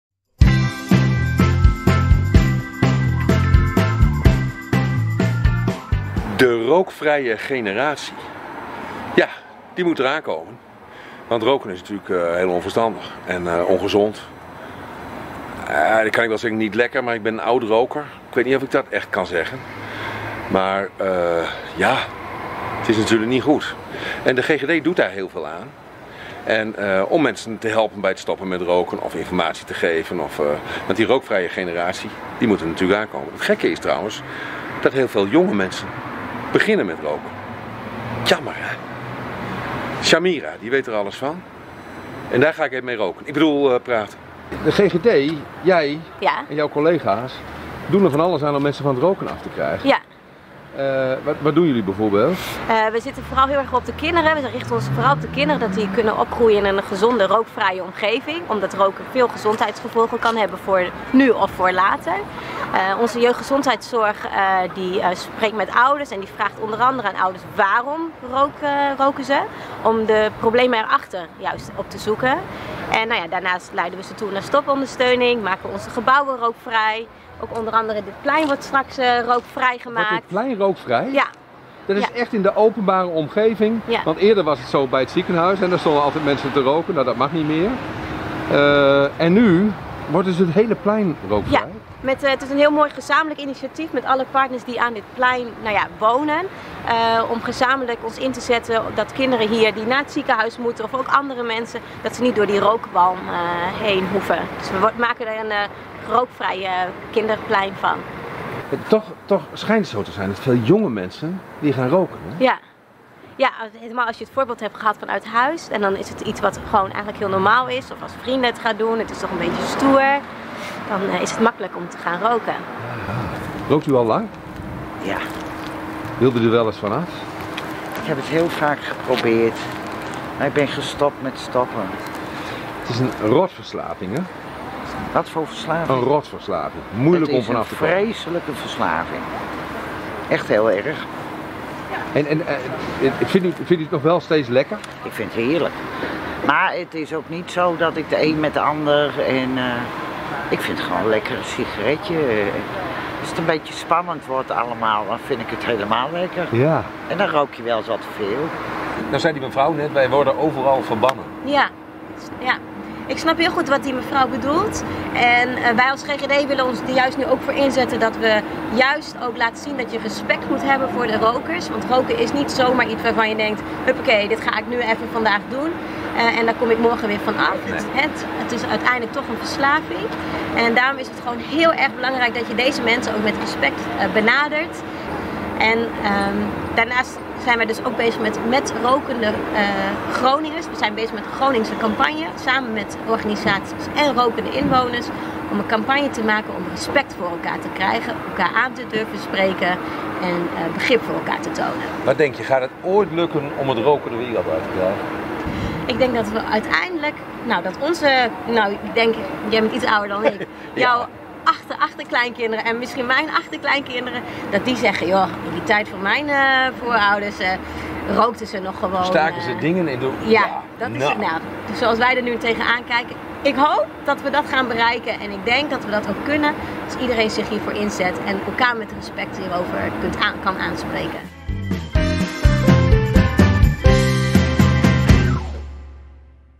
100 jaar GGD | Rookvrije generatie
GGD Groningen wordt 100 in 2019! Elke maand gaat reporter Rob Mulder voor ons op stap. Dit keer praat hij met gezondheidsbevorderaar Samirah Schipper van de GGD over de rookvrije generatie. Tijdens het gesprek hebben zij het over de gevolgen van roken, stoppen met roken en de ambitie om met z’n allen te gaan voor een nieuwe rookvrije generatie. Ook het Hanzeplein in Groningen is onderwerp van gesprek; dit plein wordt door de omliggende organisaties binnenkort als rookvrij plein aangemerkt.